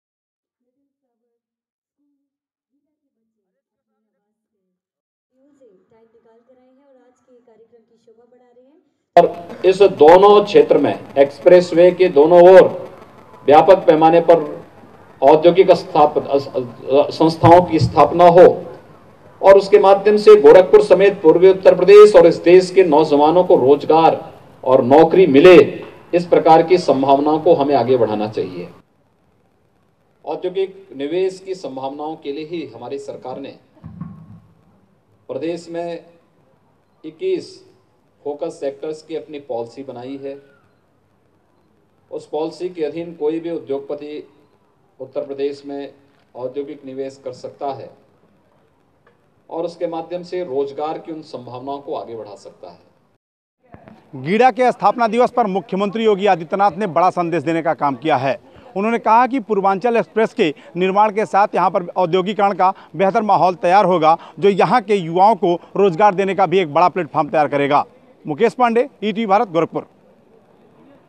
पूर्वांचल एक्सप्रेस-वे के निर्माण से तैयार होगा औद्योगीकरण माहौल : सीएम योगी
गोरखपुर : मुख्यमंत्री योगी आदित्यनाथ ने कहा कि पूर्वी उत्तर प्रदेश में उद्योग लगाने की बेहद जरूरत है. इस क्षेत्र की करीब आठ करोड़ आबादी रोजगार के लिए पलायन को मजबूर है. उन्होंने भरोसा दिलाया कि आने वाले समय में पूर्वांचल एक्सप्रेस के निर्माण से बेहतर औद्योगिक माहौल तैयार होगा. इससे युवाओं को रोजगार भी मिलेगा और यहां का माहौल भी बदलेगा.